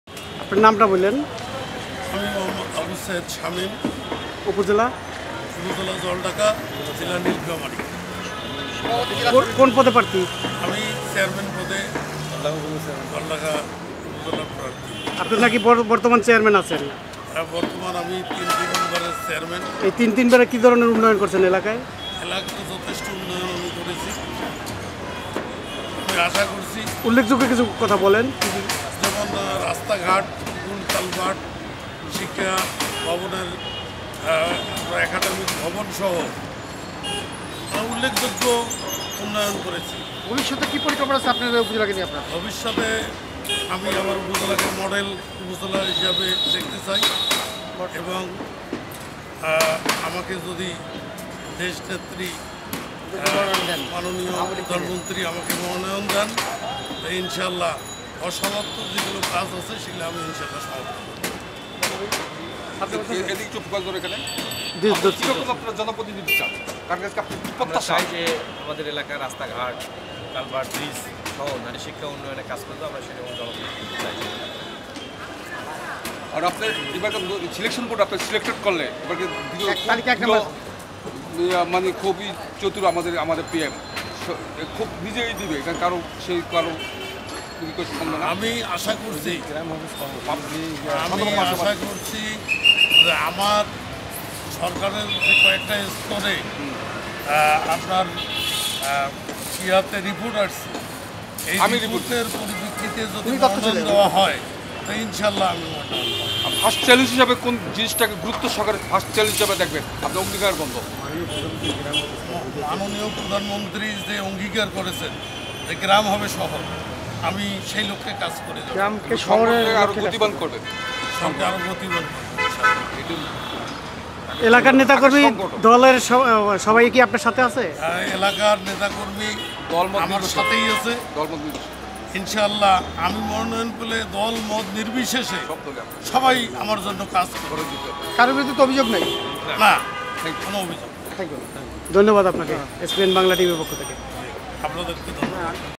জলডাঙ্গা উপজেলা চেয়ারম্যান পদ প্রার্থী
আসন্ন উপজেলা পরিষদ নির্বাচনে নীলফামারী জেলার জলডাঙ্গা উপজেলা চেয়ারম্যান পদ প্রার্থী আবু সাঈদ শামীম।